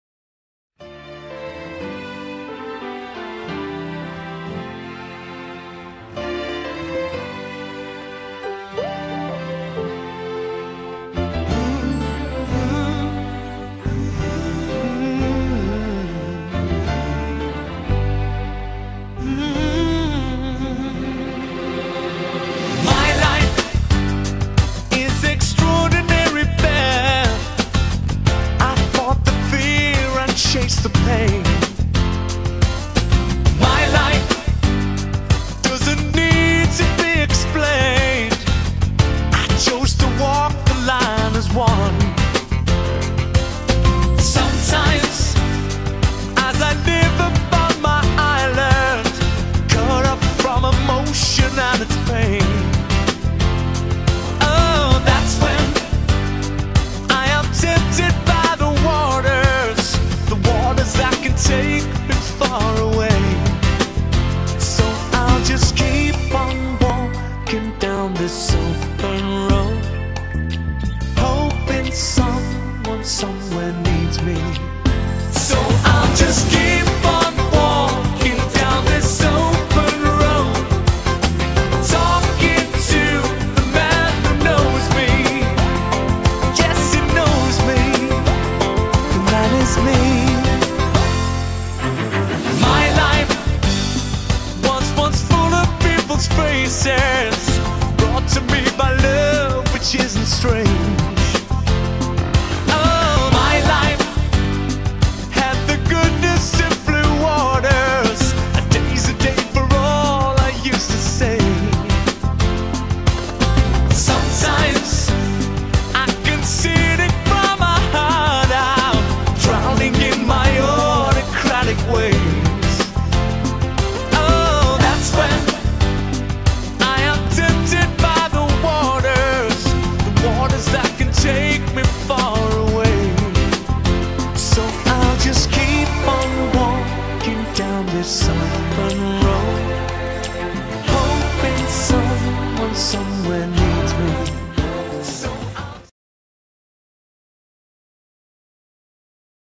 Max Hollis
A short slideshow with my Rough Collie who sadly died this year